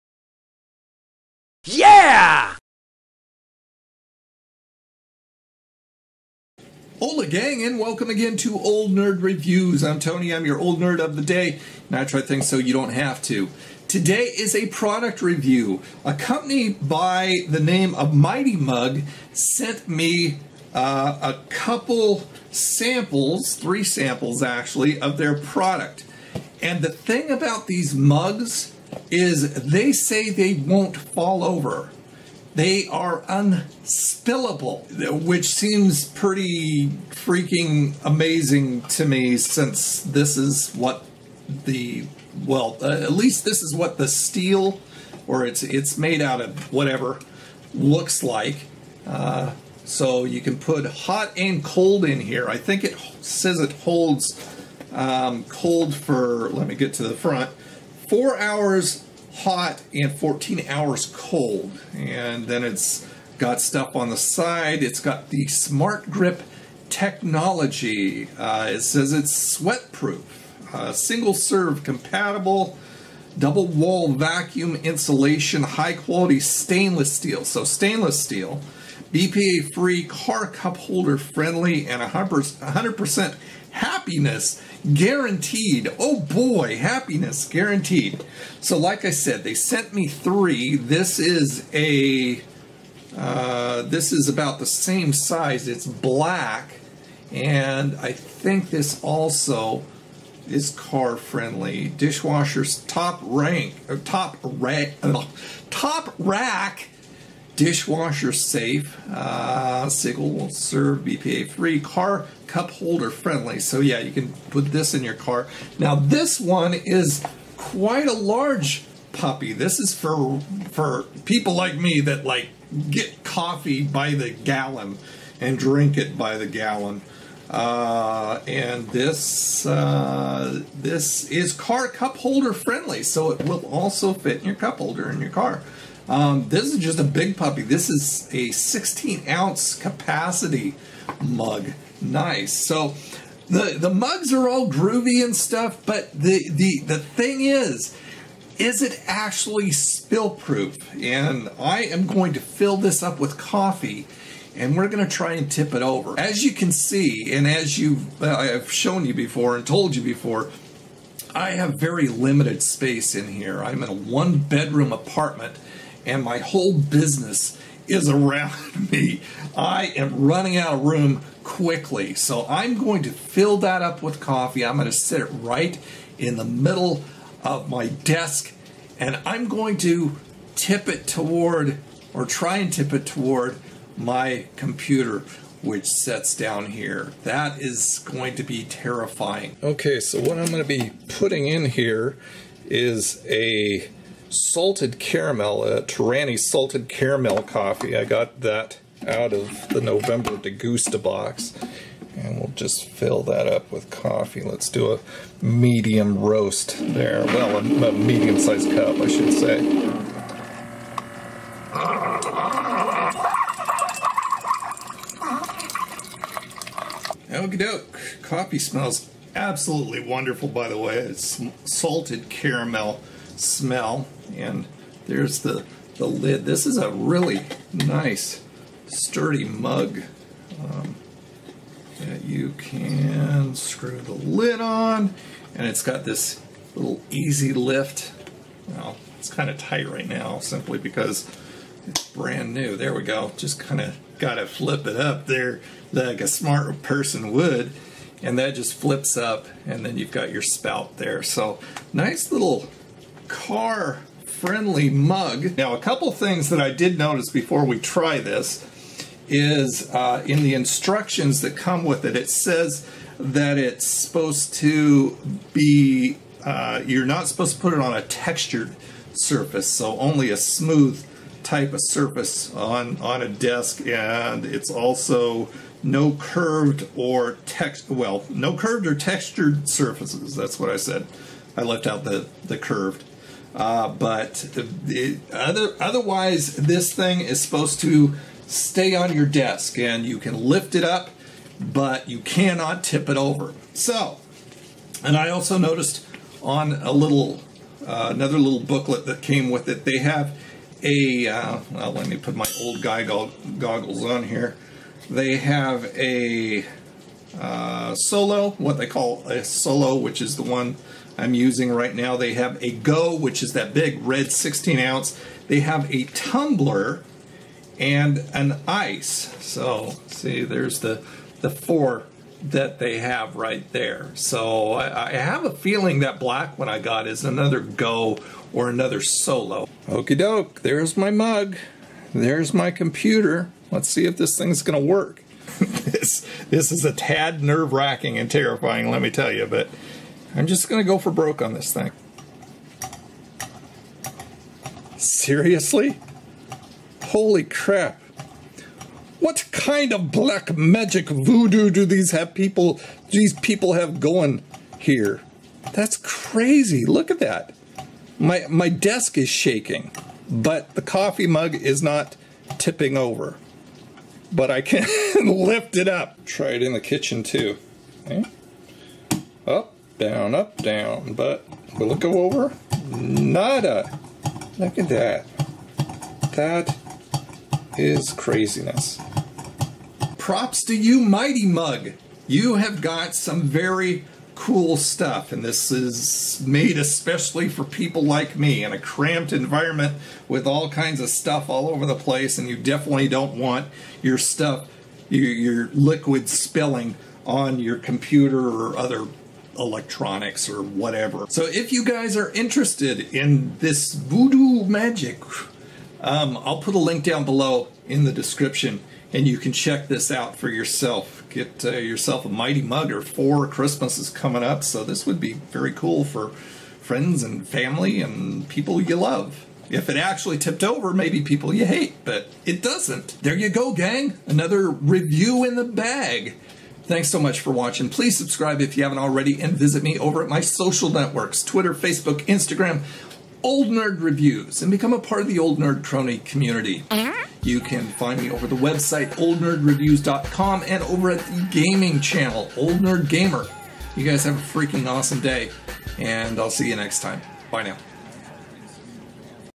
WILL IT SPILL ON MY COMPUTER? | No Spill Mighty Mug REVIEW
I'm testing out the claim that the Mighty Mug doesn't tip over and won't spill. My computer may be the victim.
⭐ BEST SHOPPING FOR NERDS & GEEKS:
👉 Great Stuff For Nerds & Geeks

My Old Nerd Reviews food review channel!! 👉